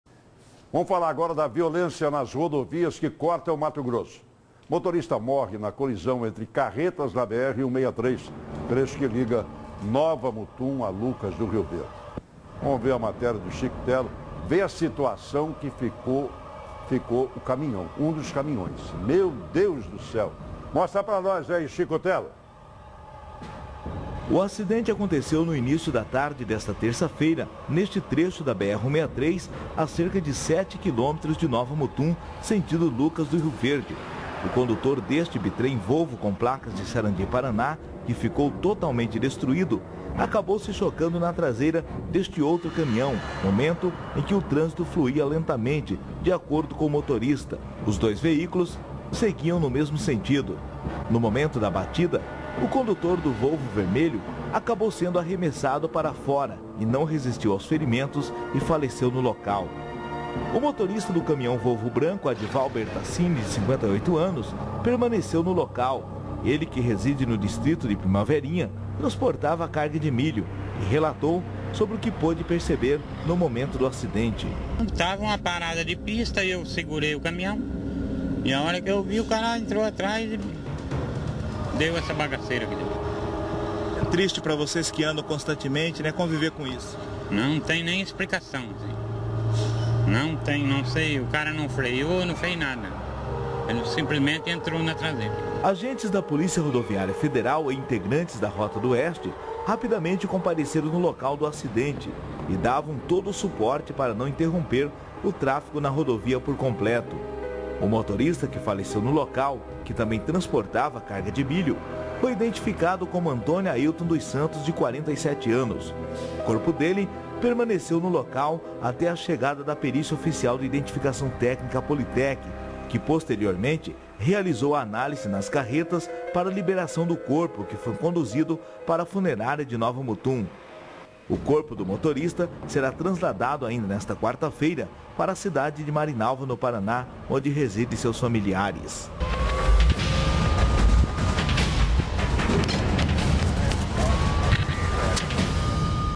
Motorista morre em acidente na BR-163
Colisão entre carretas arremessou um dos condutores que morreu na hora no trecho que liga Nova Mutum e Lucas do Rio Verde.